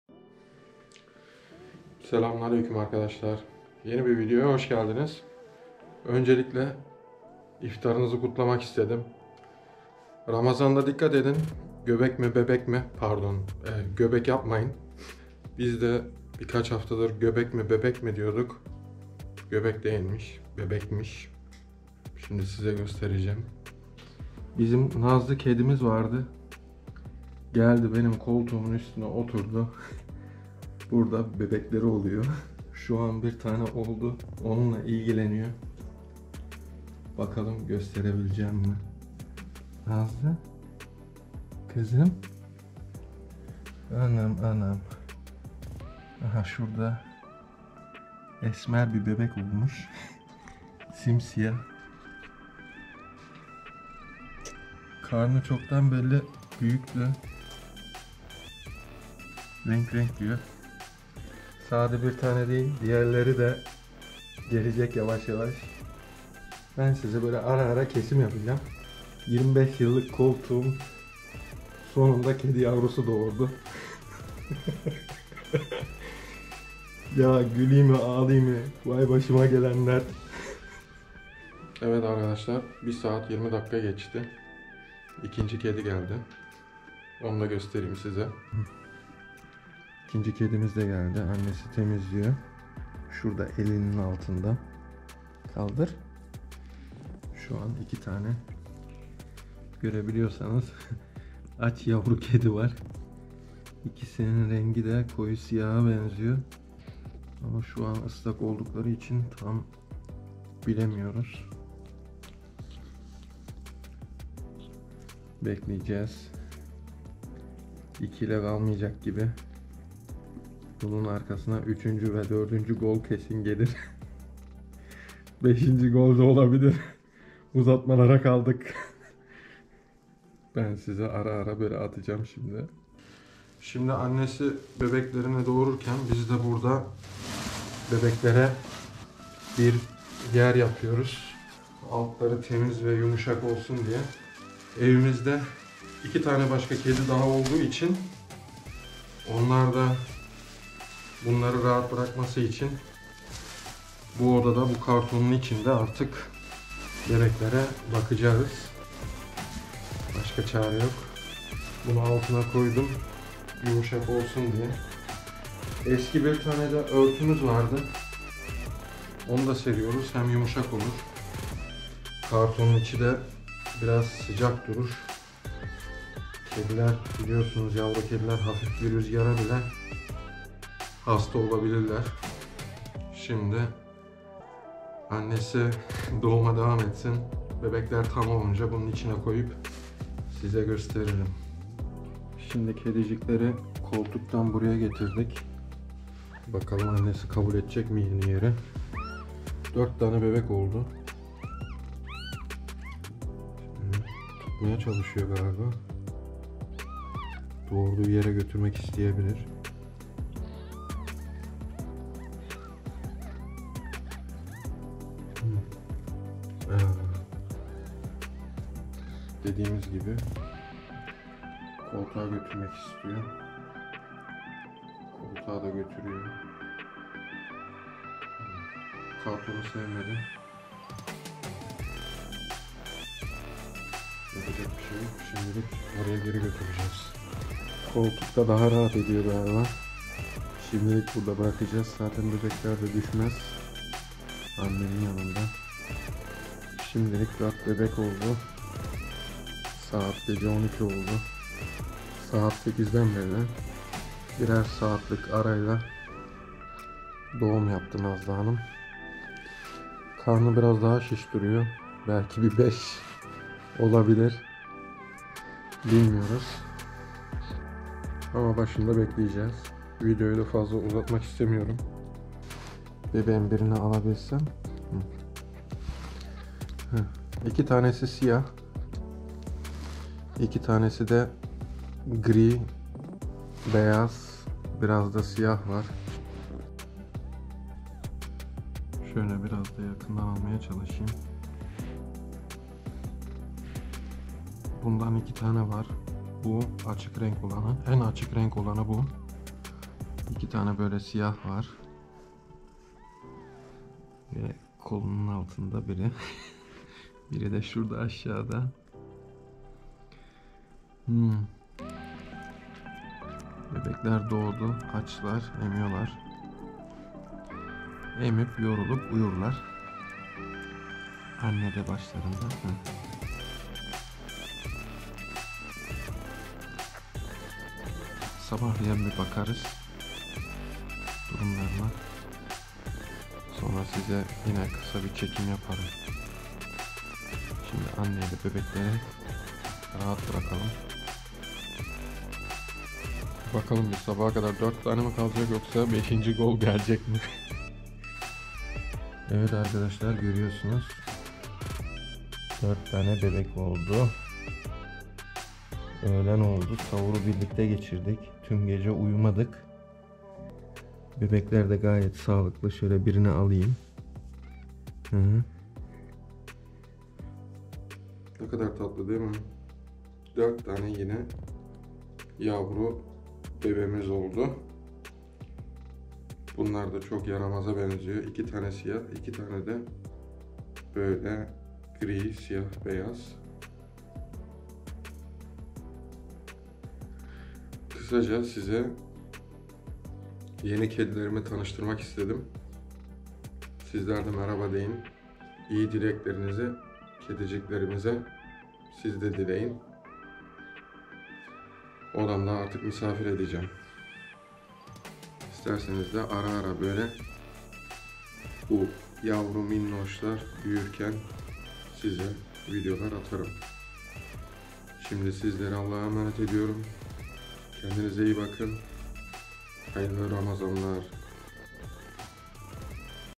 Göbekmi Bebekmi ?
Göbekmiş Bebekmiş dedi?

Haftalarca bu soru işaretiyle Ailecek Muhabbetten sonra
4 Tane yavru Bebek kedi geldi.

Ramazanın ilk Günlerinde bu şirin tatlı ziyaretçilerimizide sizlere tanıştıralım dedik .